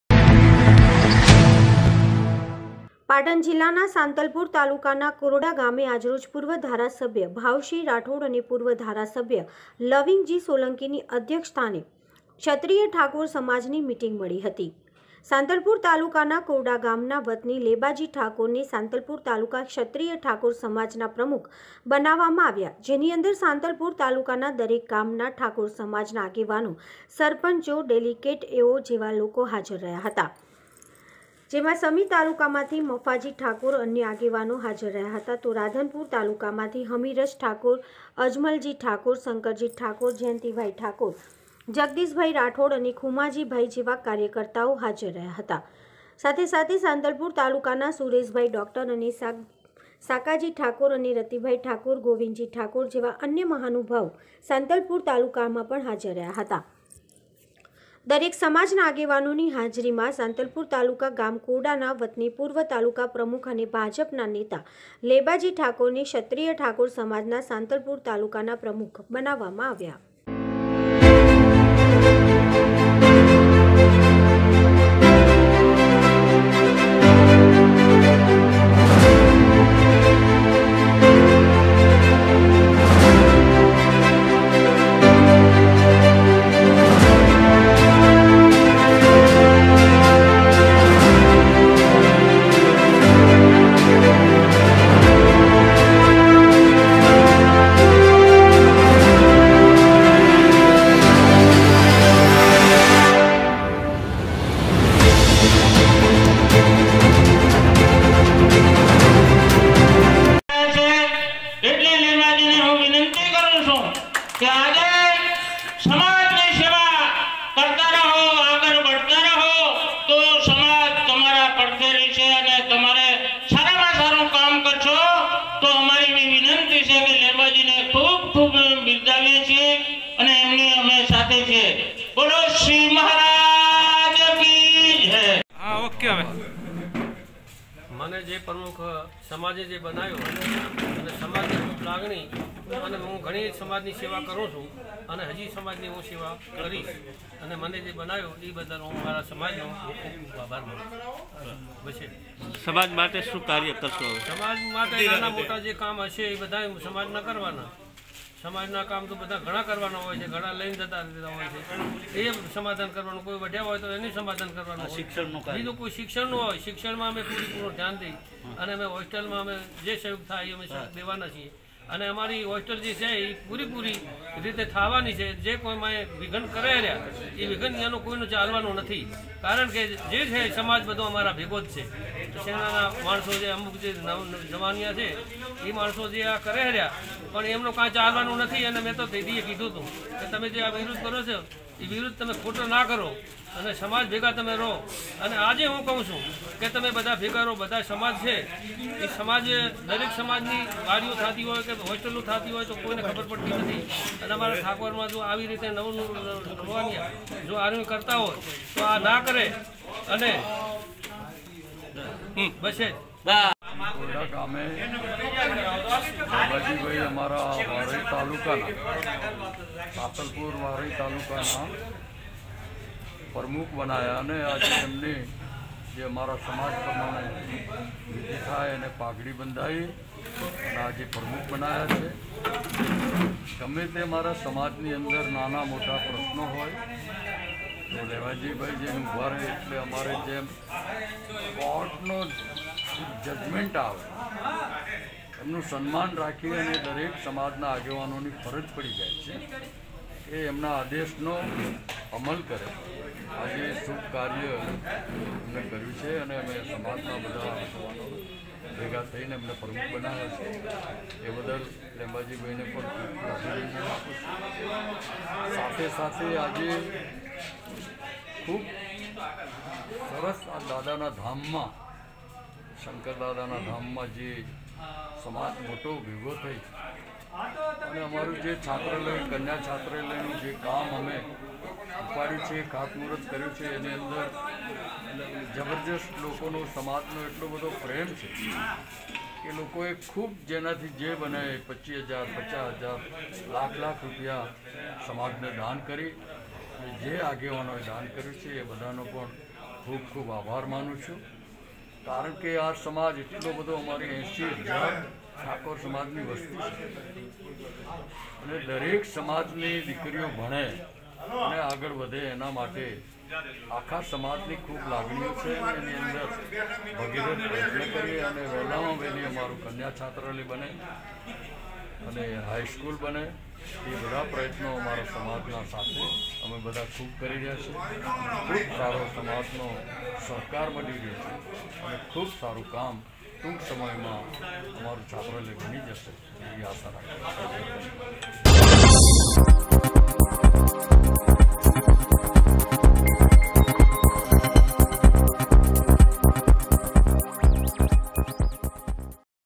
પાટણના સાતલપુર તાલુકા ના કોરડા ગામે ક્ષત્રિય ઠાકોર સમાજની મિટીંગ.....
પાટણ જિલ્લાના સાતલપુર તાલુકા ના કોરડા ગામે આજરોજ પૂર્વ ધારાસભ્ય ભાવસિંહ રાઠોડ અને પૂર્વ ધારાસભ્ય લવિંગજી સોલંકી ની અધ્યક્ષ સ્થાને ક્ષત્રિય ઠાકોર સમાજની મિટીંગ મળી હતી gem સાતલપુર તાલુકા ના કોરડા ગામ ના આ વતની લેબાજી ઠાકોર ને સાતલપુર તાલુકા ક્ષત્રિય ઠાકોર સમાજના પ્રમુખ બનાવવામાં આવેલા જેની અંદર સાતલપુર તાલુકા ના દરેક ગામના ઠાકોર સમાજના આગેવાનો સરપંચો ડેલિકેટ ઓ જેવા લોકો હાજર રહેલા જેમાં સમી તાલુકામાંથી મફાજી ઠાકોર અને અન્ય આગેવાનો હાજર રહેલા તો રાધનપુર તાલુકા માંથી હમીરજી ઠાકોર અજમલજી ઠાકોર શંકરજી ઠાકોર જયંતીજી ઠાકોર જગદીશભાઈ રાઠોડ અને ખુમાજી ભાઈ જેવા કાર્યકર્તાઓ હાજર રહેલા સાથે સાથે સાતલપુર તાલુકા ના  સુરેશભાઈ ડોક્ટર અને શાકાજી ઠાકોર અને રતિલાલ ઠાકોર ગોવિંદજી ઠાકોર જેવા અન્ય મહાનુભવો સાતલપુર તાલુકા ના પણ હાજર રહેલા દરેક સમાજના આગેવાનોની હાજરીમાં સાતલપુર તાલુકા ગામ કોરડા ના વતની પૂર્વ તાલુકા પ્રમુખ અને ભાજપના નેતા લેબાજી ઠાકોરને ક્ષત્રિય ઠાકોર સમાજના સાતલપુર તાલુકા ના પ્રમુખ બનાવવામાં આવ્યા.